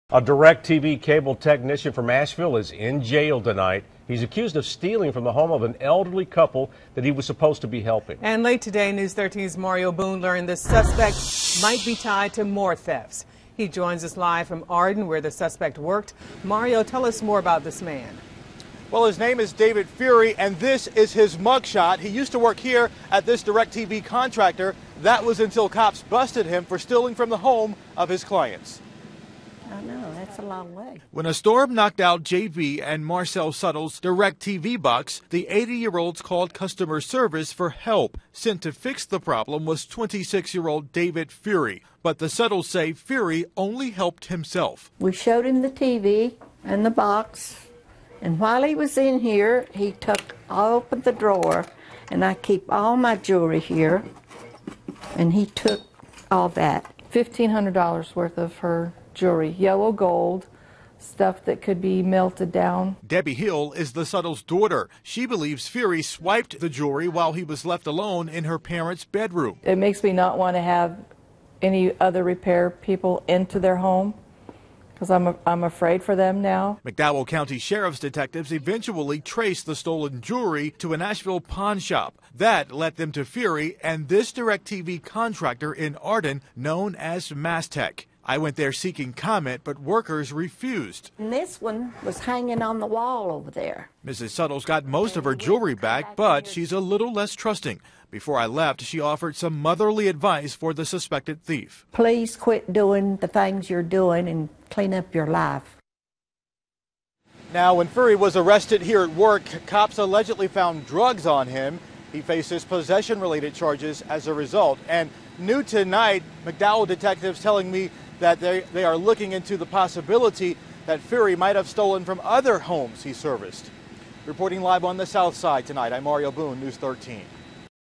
Cops: DIRECTV Repairman Ripped Off Clients
When a storm knocked out J.B. and Marcelle Suttles' DIRECTV box the 80-year-olds called customer service for help.  Sent to fix the problem was 26-year old David Fuhry.  But the Suttles say Fuhry only helped himself.

We showed him the TV, said Mrs. Suttles.  While he was in here he opened the drawer where I keep all my jewelry and he took all that.  She estimated $1,500 worth gold bracelets and a diamond ring were stolen.

Debbie Hill is the Suttles' daughter.  She told News 13's Mario Boone Fuhry swiped the jewelry while he was left alone in her parents' bedroom.  It makes me not want to have any other repair people in their home because I'm afraid for them now, said Hill.

McDowell County sheriff's detectives eventually traced the stolen jewelry to an Asheville pawn shop.  That led them to Fuhry and his employer, DIRECTV contractor known as Mastec.

Boone went there Friday seeking comment but workers refused.

Mrs. Suttles got most of her jewelry back but she's a little less trusting.  And she offered some motherly advice for the suspected thief.  Please quit doing the things you're doing and clean up your life, she said.

Follow on Twitter: @marioboone